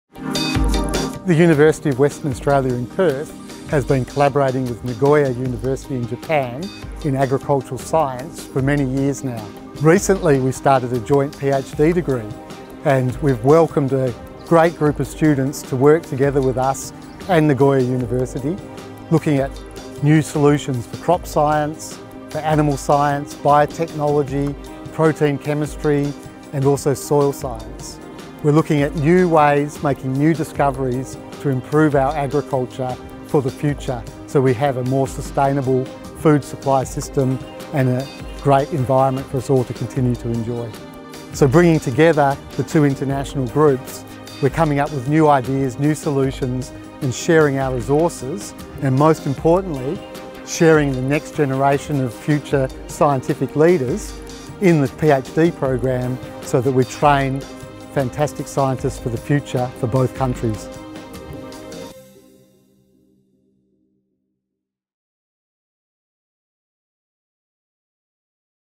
First crop of agriculture students from Japan join UWA
UWA Deputy Vice-Chancellor (Research) Professor Timothy Colmer talks about the joint PhD program he established between UWA and Japan's Nagoya University.